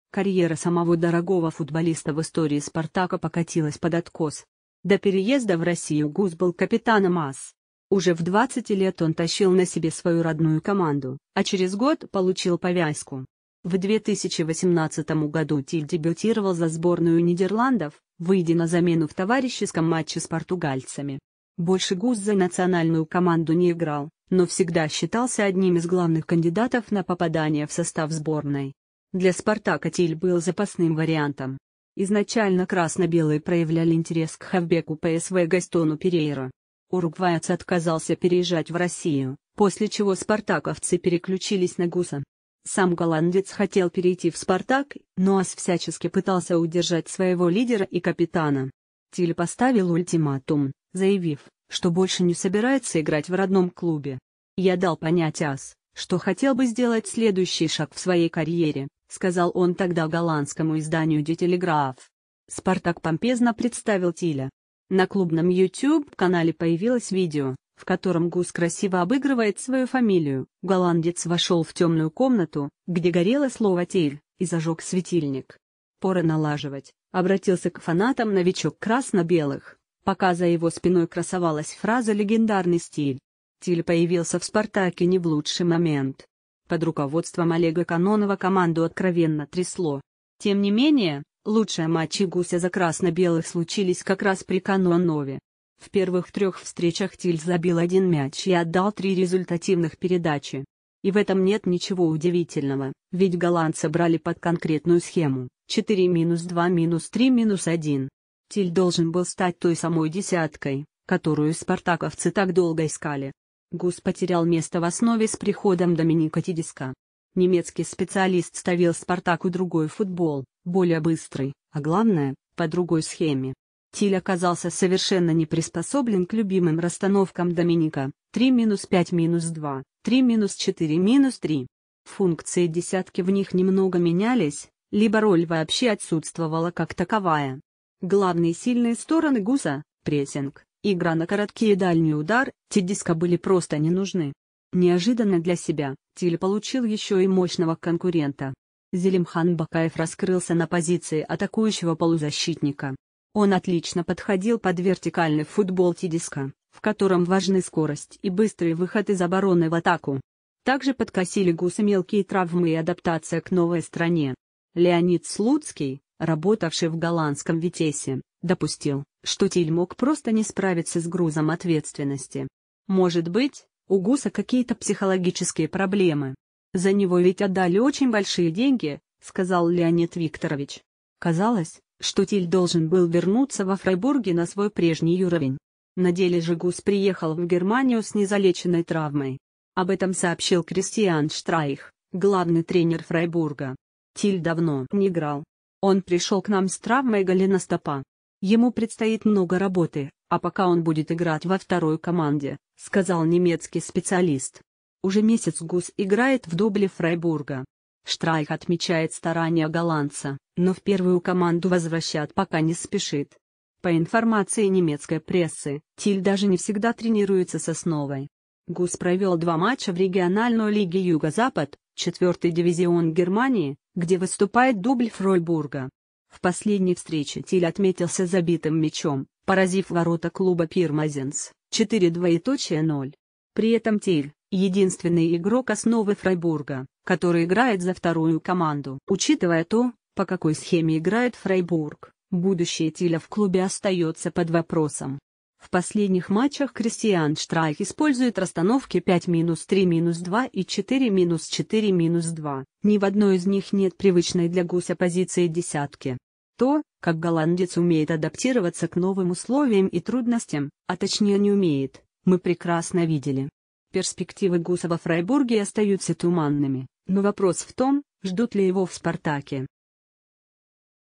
Вы ещё помните Гуса Тиля? Сейчас он играет за дубль аутсайдера Бундеслиги
Карьера самого дорогого футболиста в истории «Спартака» покатилась под откос.